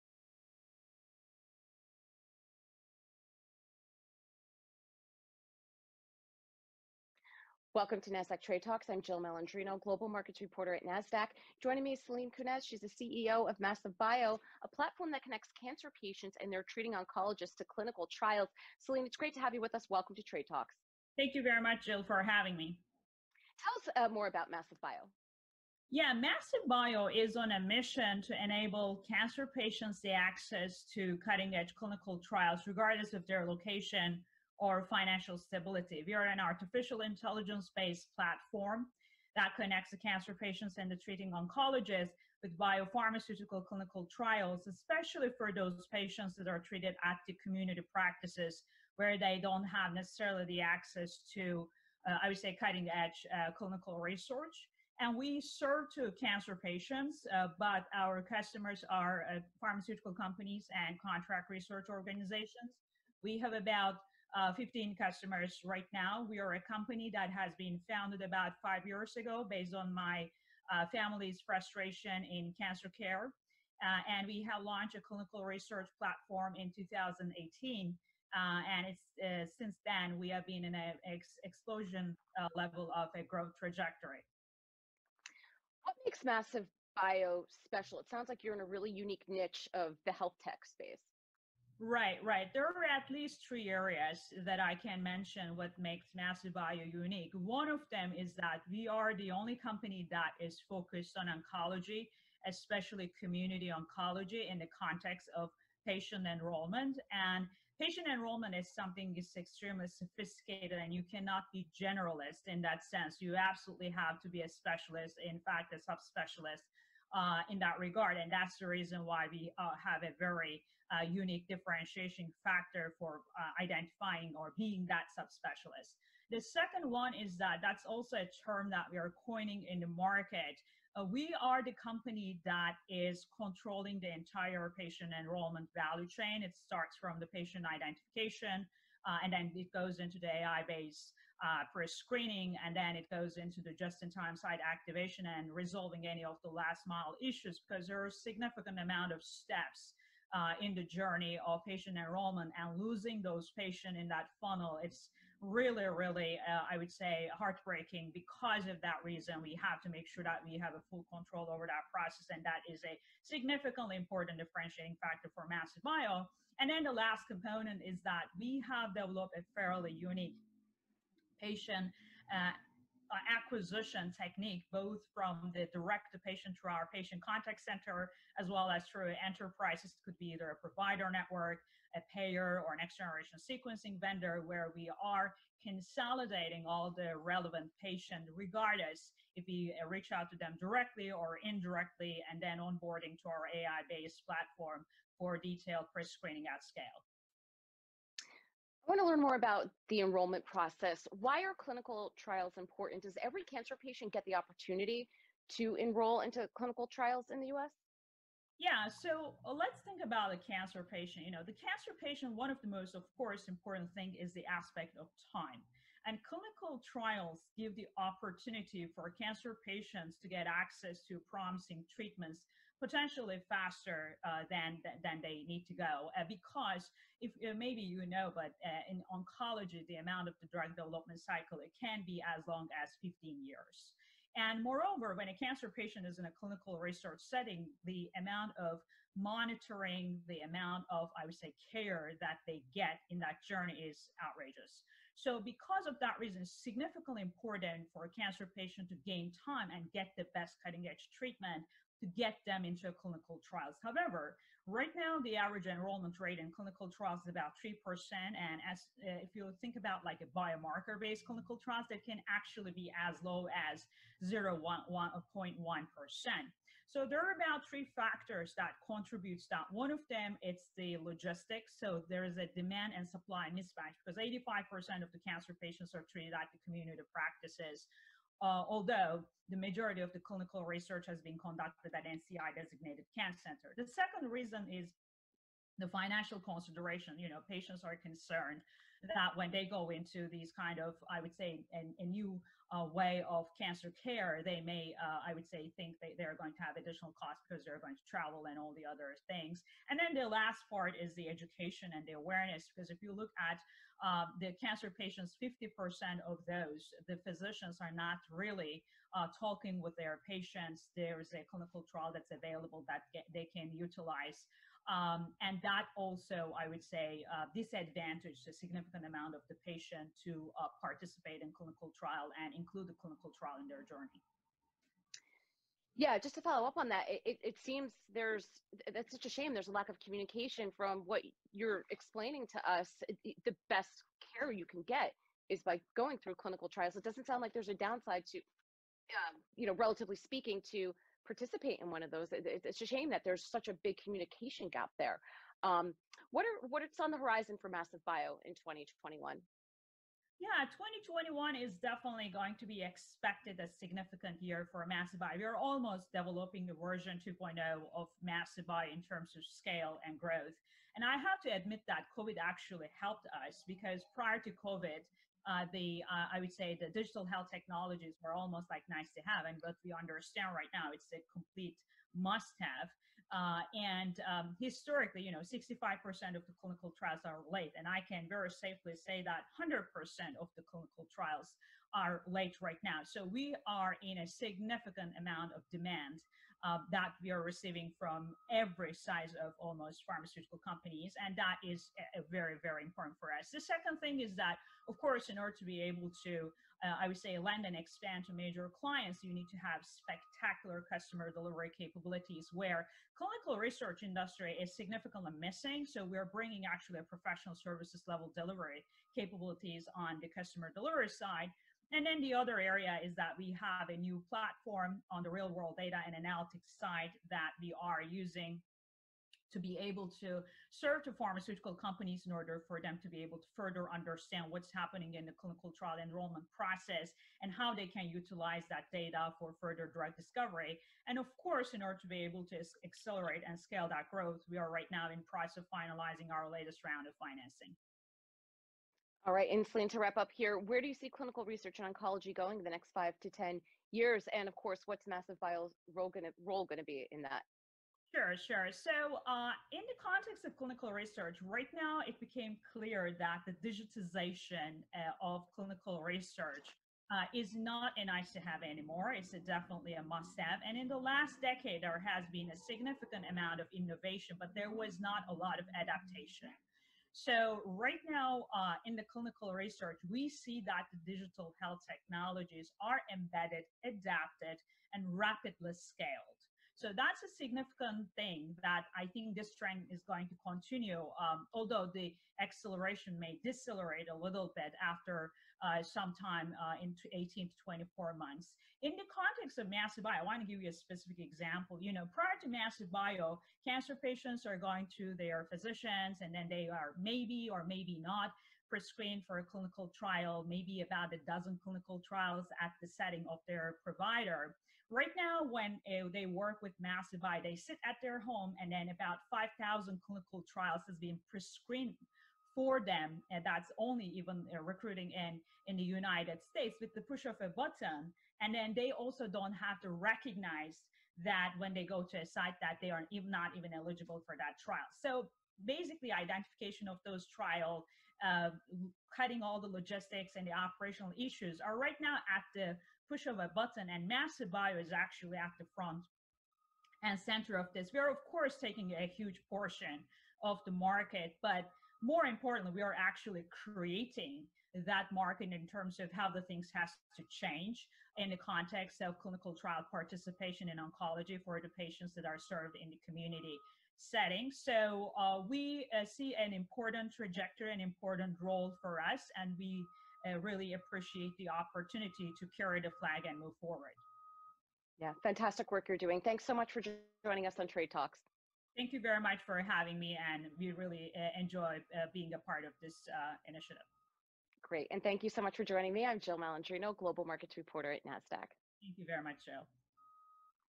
Selin Kurnaz: Connecting Cancer Patients to Clinical Trials on NASDAQ
Massive Bio CEO Selin Kurnaz joins Nasdaq's TradeTalks with Jill Malandrino to discuss connecting cancer patients to clinical trials using Massive Bio's proprietary Artificial Intelligence powered database, the SYNERGY AI: Tumor Registry.